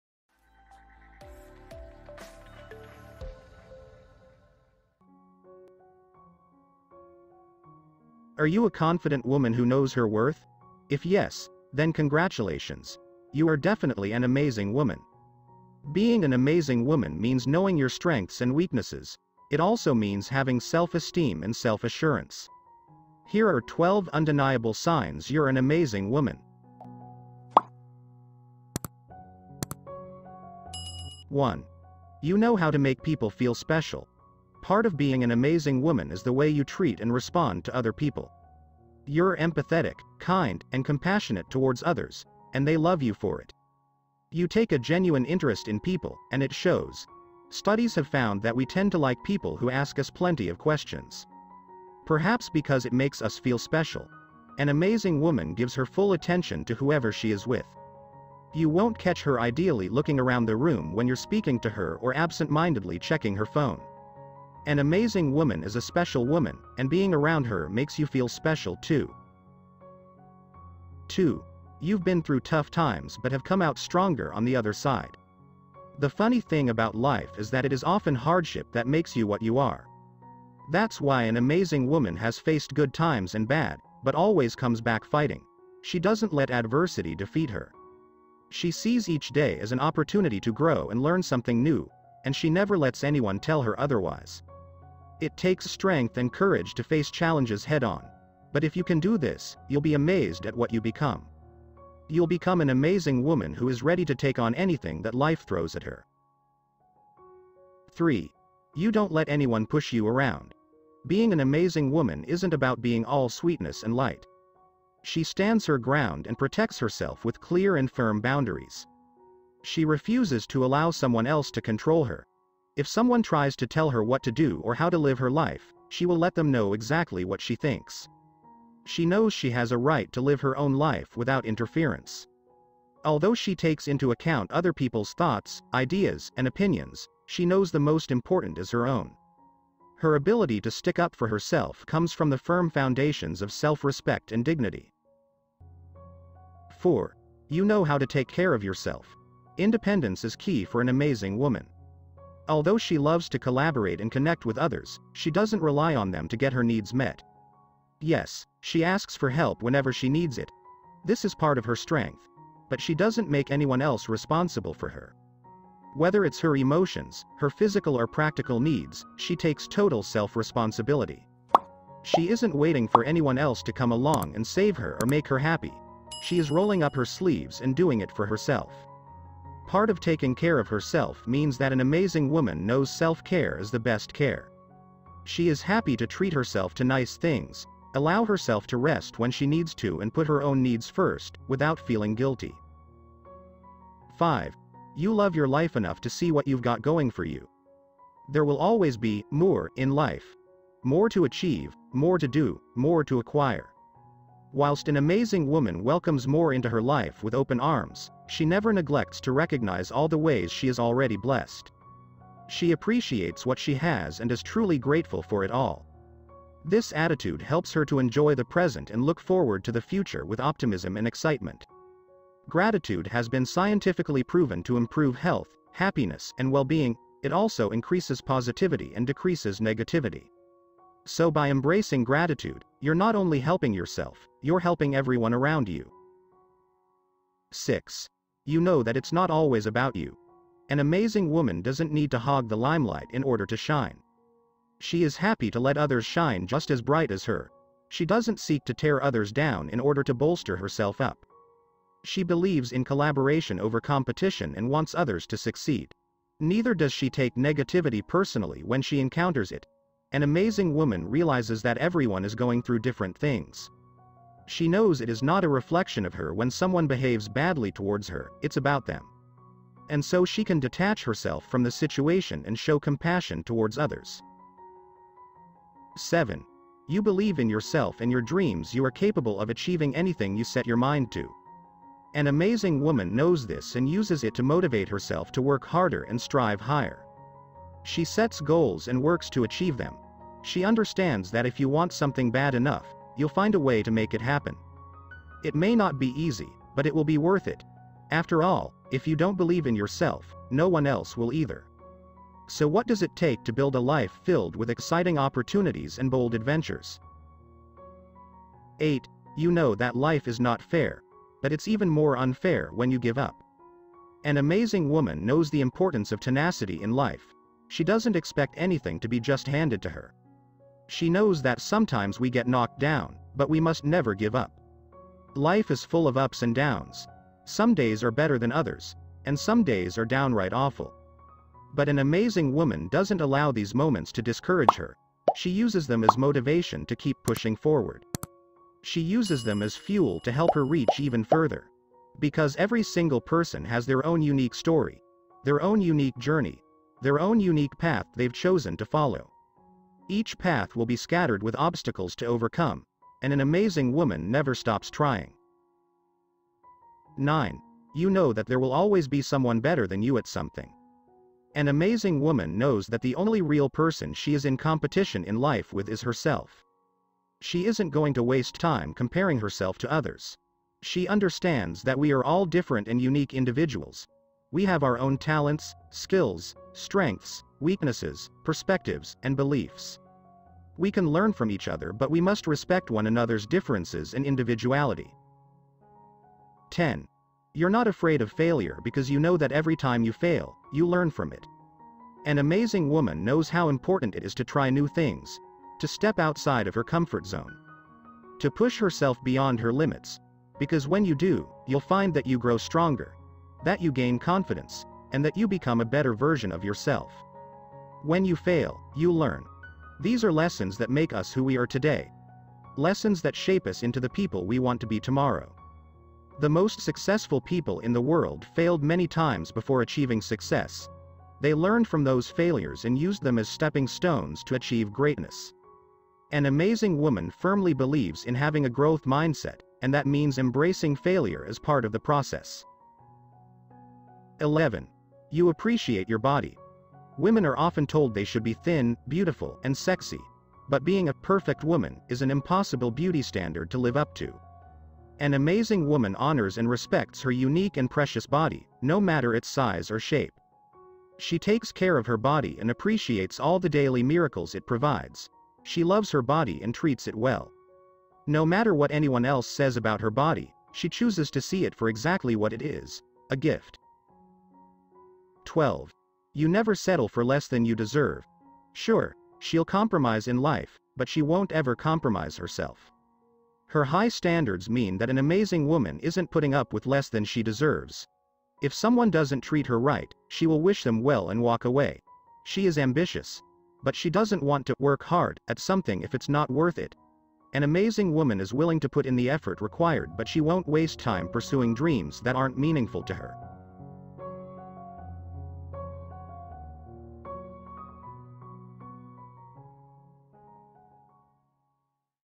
12 undeniable signs you’re an amazing woman
Are you a confident woman who knows her worth? If yes, then congratulations. You are definitely an amazing woman.
Being an amazing woman means knowing your strengths and weaknesses. It also means having self-esteem and self-assurance.
Here are 12 undeniable signs you’re an amazing woman.

1) You know how to make people feel special.
2) You’ve been through tough times but have come out stronger on the other side
3) You don’t let anyone push you around
4) You know how to take care of yourself
5) You love your life enough to see what you’ve got going for you
6) You know that it’s not always about you
7) You believe in yourself and your dreams
8) You know that life is not fair. But it’s even more unfair when you give up.
9) You know that there will always be someone better than you at something
10) You’re not afraid of failure because you know that every time you fail, you learn from it
11) You appreciate your body
12) You never settle for less than you deserve